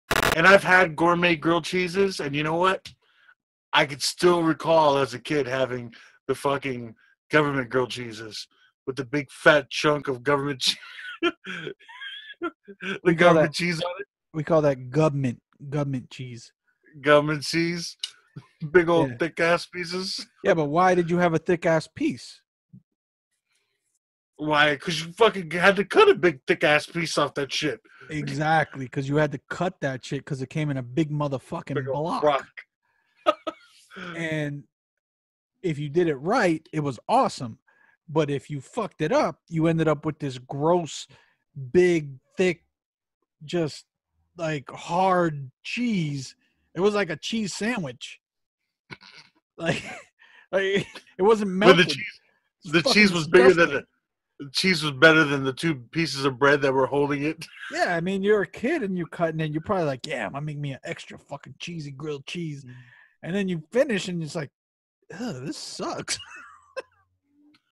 Random Richards: Gubment Grilled Cheese
Rob is not a fan of the fancy schmanzy "gourmet" grilled cheese.